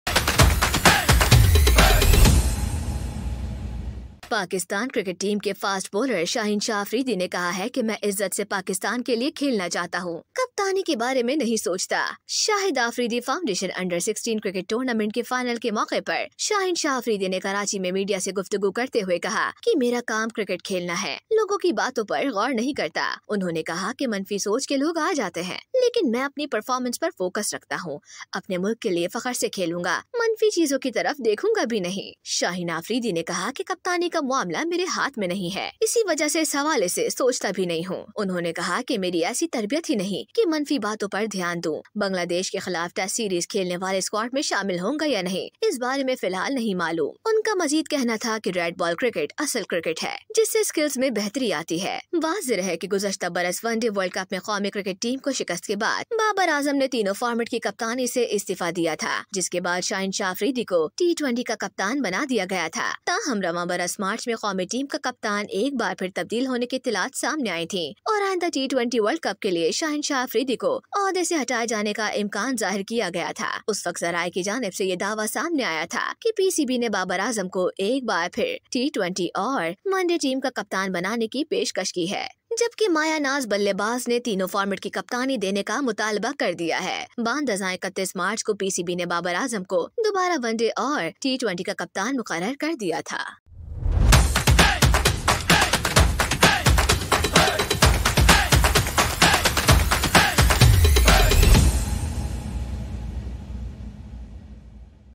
Shaheen Shah Afridi Said, I Play For Pakistan, Not For Captaincy | Hungama Express
Shaheen Shah Afridi states that he plays to serve Pakistan, not for captaincy. His statement has won the hearts of cricket fans.